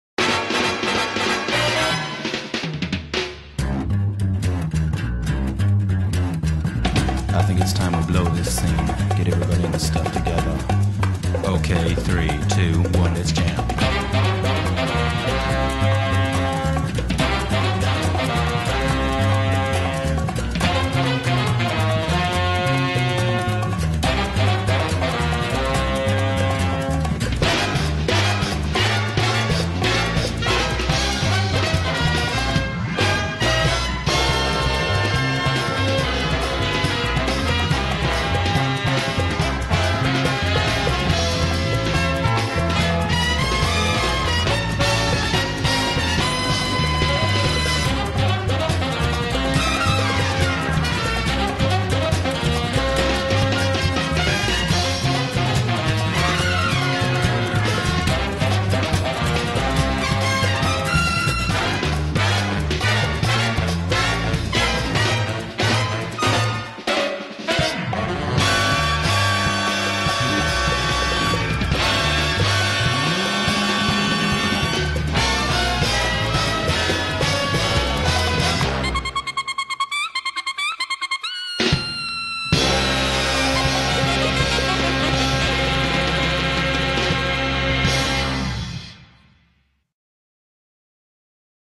Ed (Cowboy Bebop) - A Timelapse Video
Maybe not everyone's favorite character from Cowboy Bebop and not mine XD But still love her! Thought Id experiment a little with the watercolor but It didn't turn out as I would of liked it too! Still had fun with it though and at the end of the day what else matters? See you space cowboy..